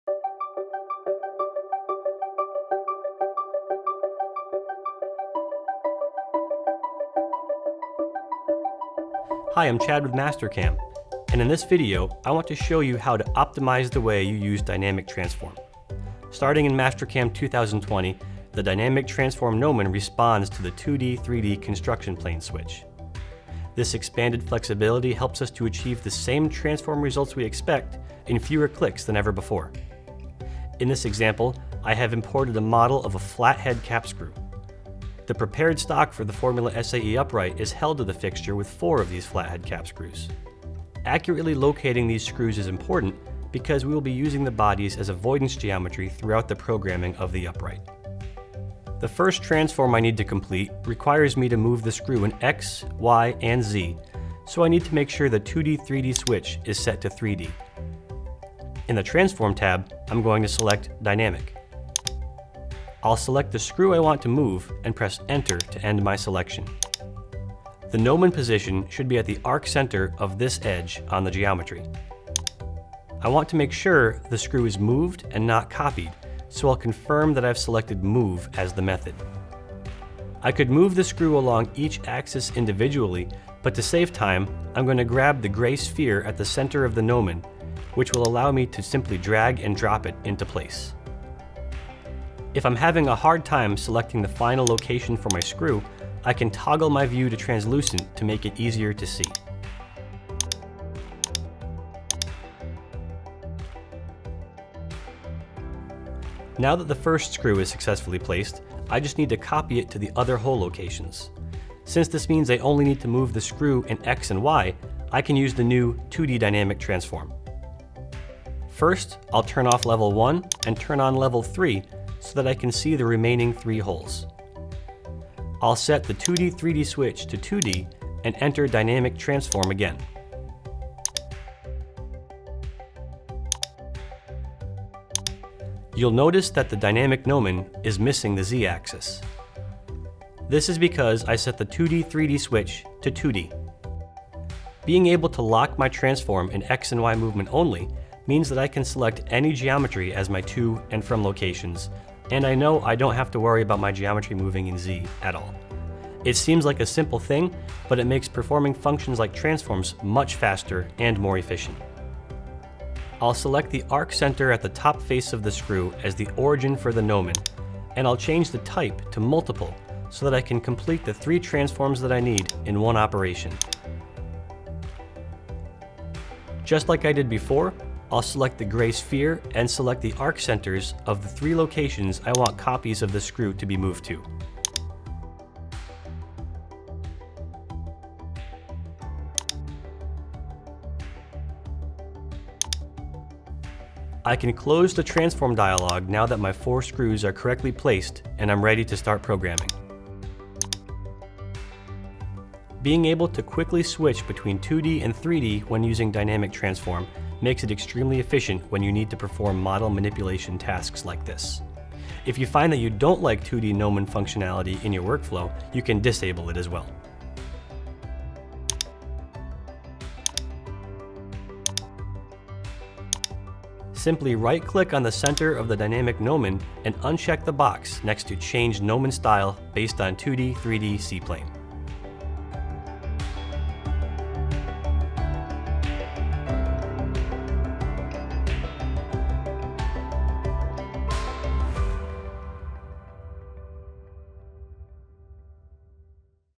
FSAE Upright Series: Dynamic Gnomon
In this video, Chad will help you optimize the way you use Dynamic Transform.